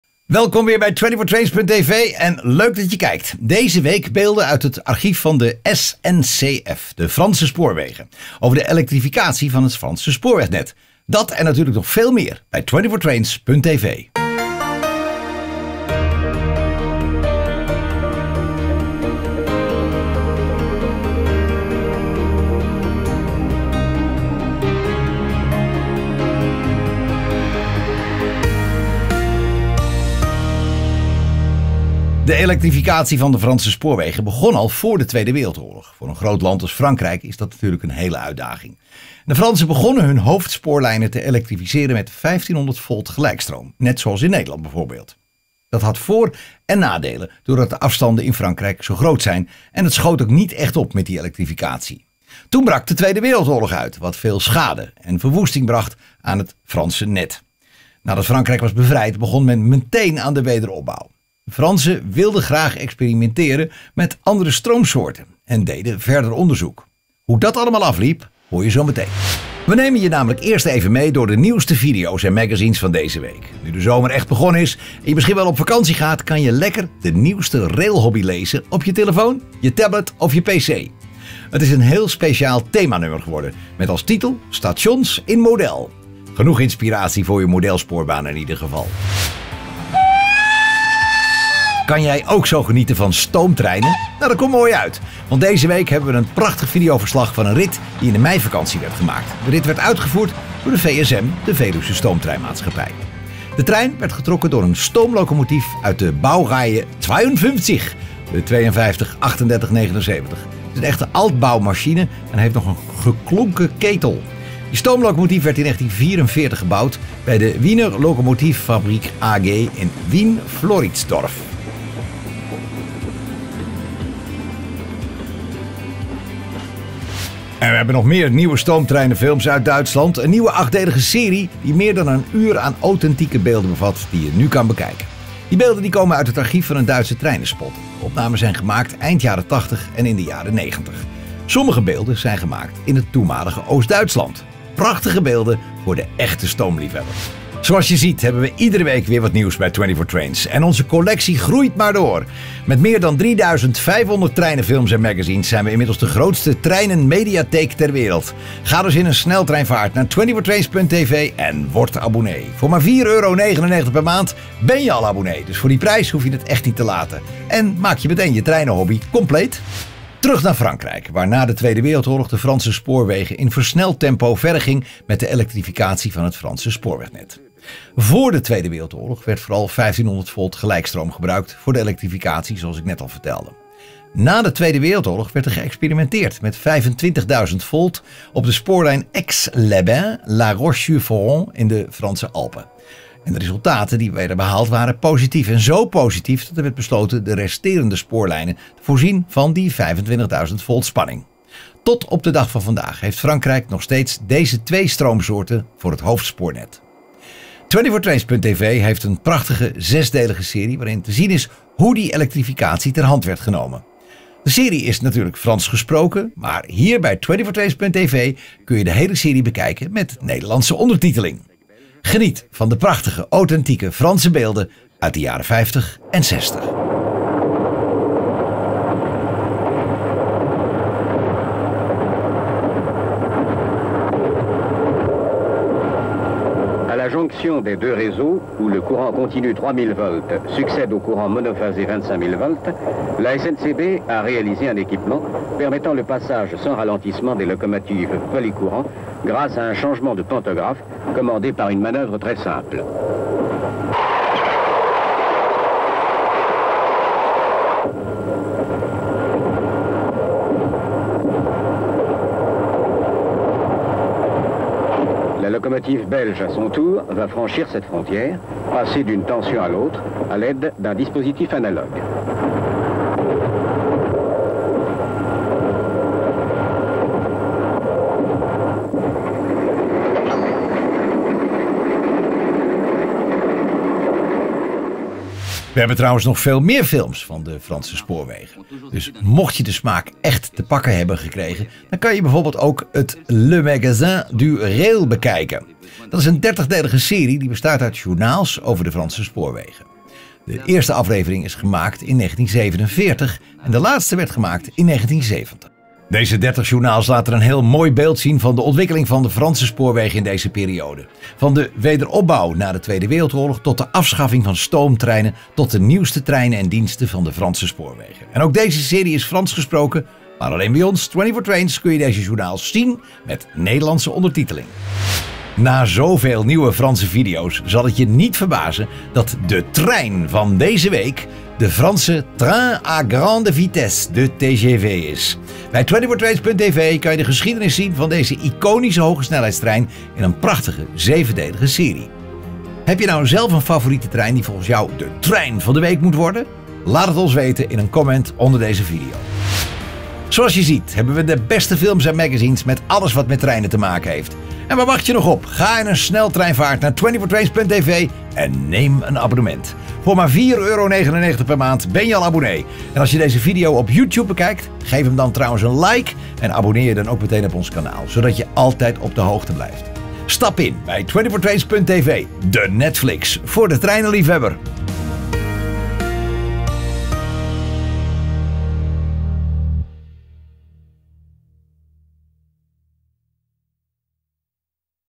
De elektrificatie van de Franse spoorwegen (SNCF) | The electrification of the French railways
Deze week kun je onder andere genieten van mooie beelden vanuit de archieven van de Franse spoorwegen!

Deze serie gaat over de elektrificatie van het spoorwegen netwerk. De opbouw hiervan begon al voor de tweede wereldoorlog en voor een groot land als Frankrijk is dit natuurlijk een hele uitdaging. De oorlog bracht veel schade en verwoestingen met zich mee. Nadat Frankrijk was bevrijd, begon men meteen met de wederopbouw. De Fransen wilde graag experimenteren met een andere stroomsoort en deed verder onderzoek. Bekijk nu hoe dat verder afliep… Dat en nog veel meer!

This week you can enjoy beautiful footages from the archives of the French railways!

This series is about the electrification of the railway network. The electrification  of this already started before the second world war and for a large country like France this is of course quite a challenge. The war caused much damage and destruction. After France was liberated, they continued immediately. The French wanted a different type of current and did further research. Now see how that turned out…

So, this week you can enjoy beautiful images from the archives of the French railways! That and much more!

Go as fast as a TGV to 24Trains.tvand become a subscriber!